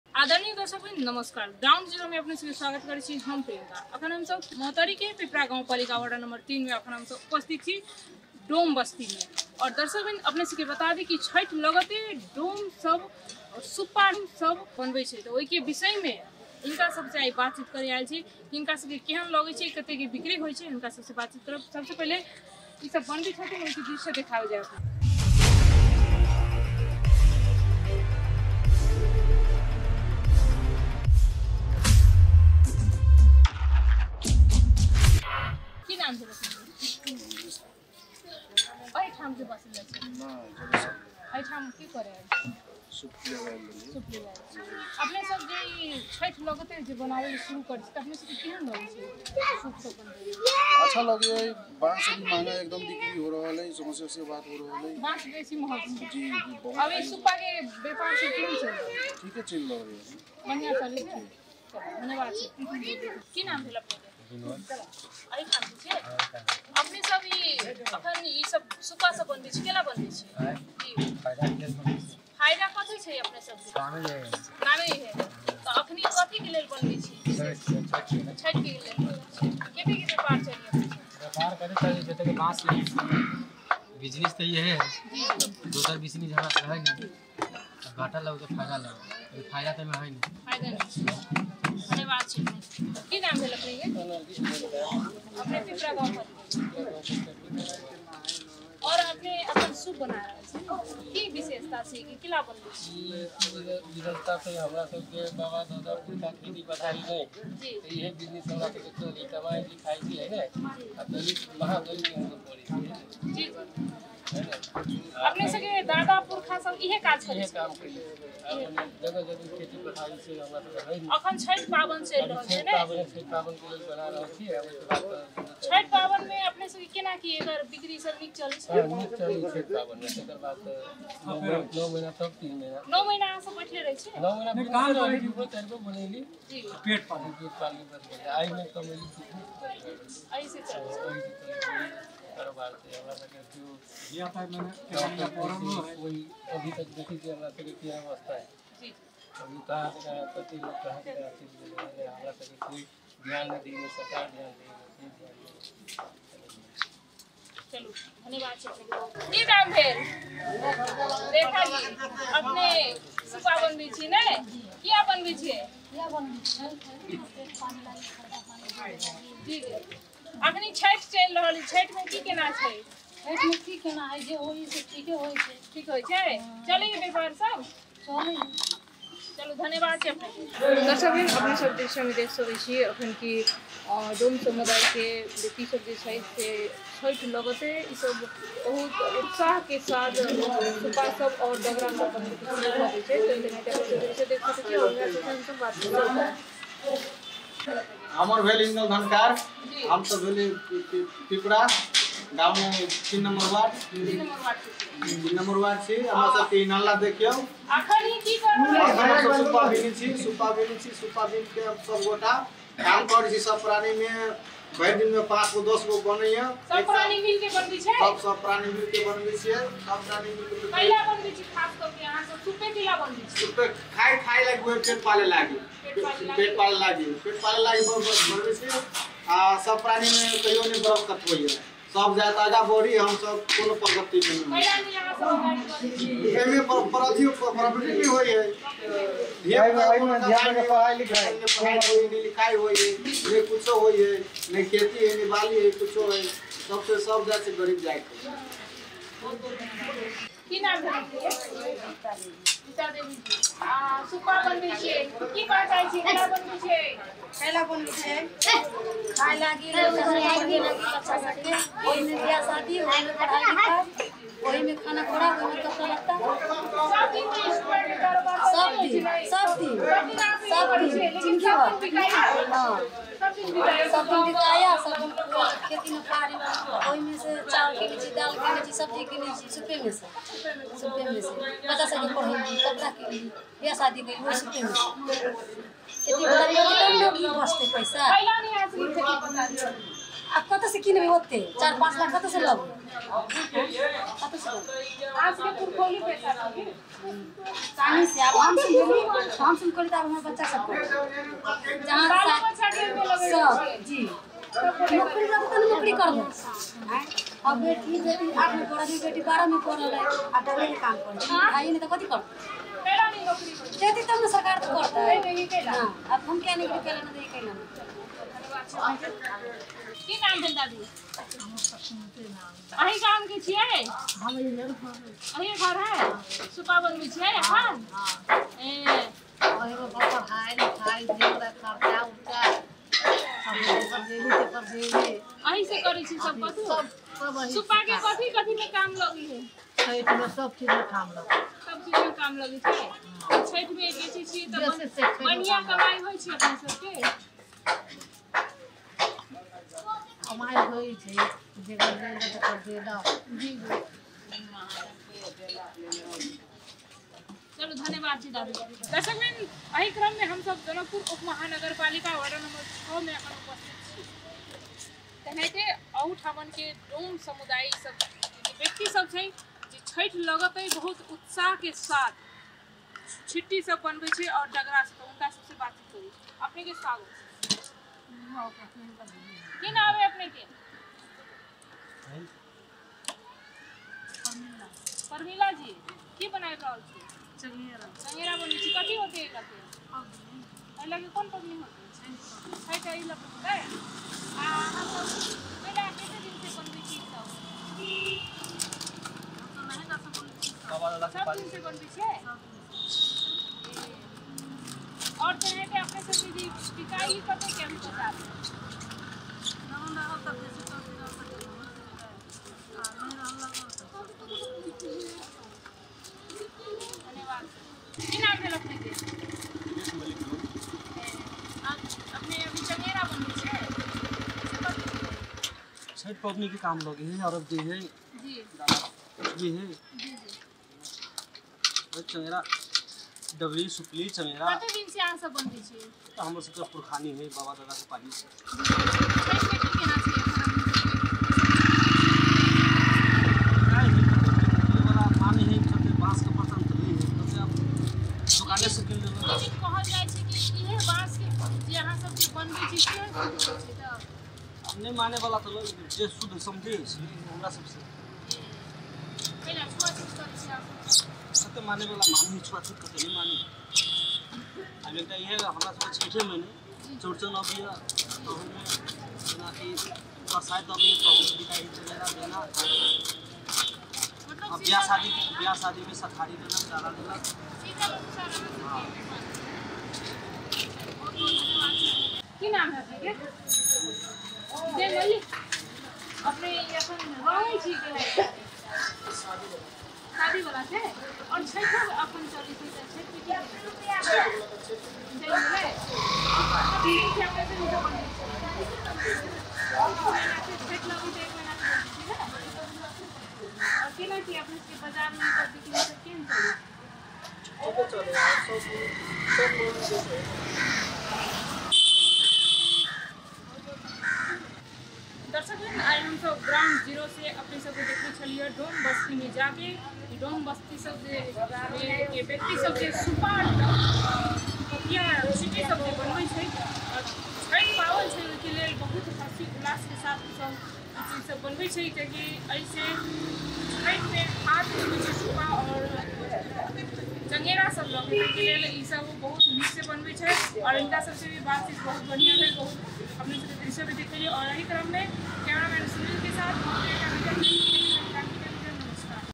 पूजाको सामग्री बनाएर आम्दानी गर्दै डोम समुदाय महोतरीको पिपरा गाउँपालिका डोम बस्ती बारेमा एक
पूजाको सामग्री बनाएर आम्दानी गर्दै डोम समुदाय महोतरीको पिपरा गाउँपालिका डोम बस्तीमा छठ पर्वको लागि नाङ्लोहरु बनाउदै त्यसको महत्व के छ र कतिको बिक्रि भईरहेको बारेमा एक रिपोर्ट .